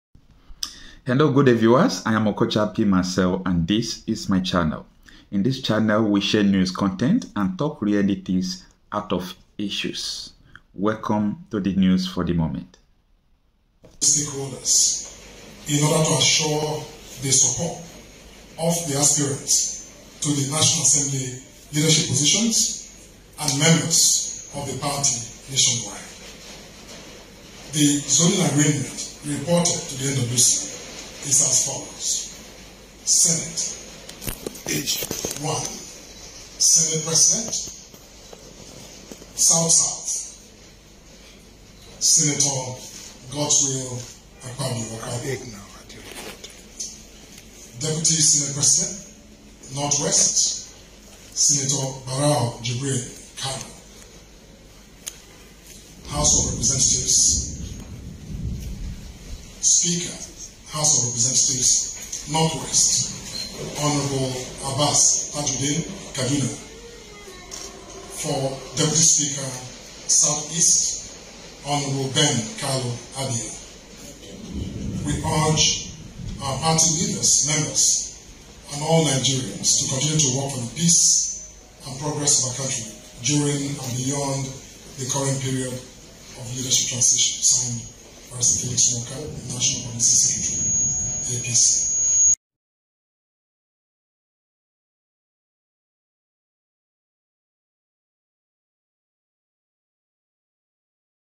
(Incase you missed this) APC announces New Senate President, Will it Work? | Nigeria News Today
Nigeria news# Latest news# Senate President